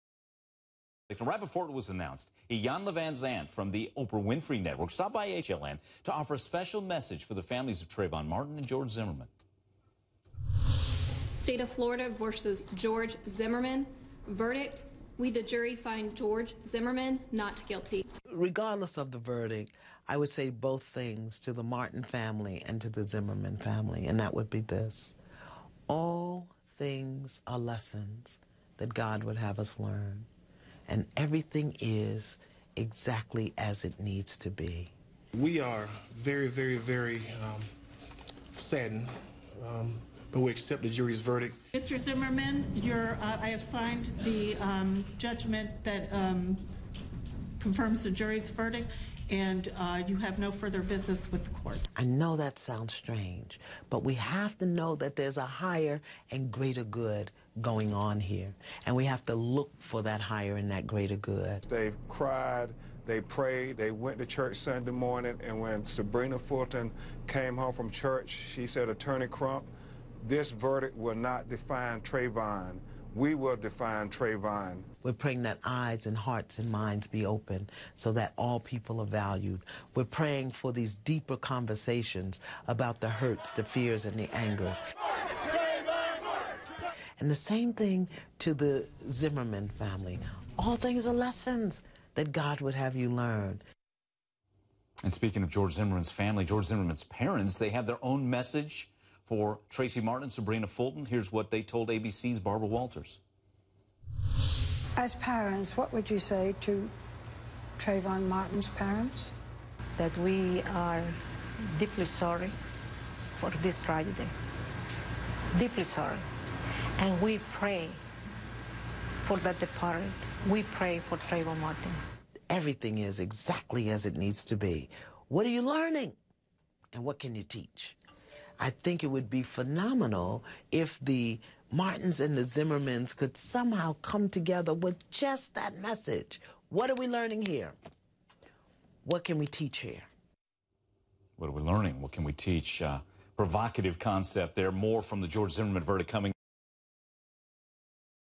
Vanzant on post-verdict healing: Learn from this
Prior to the verdict that George Zimmerman is not guilty in the killing of an unarmed teen, Iyanla Vanzant, host of "Iyanla: Fix My Life," stopped by HLN to offer a message of healing to the parents and loved ones of Trayvon Martin and Zimmerman.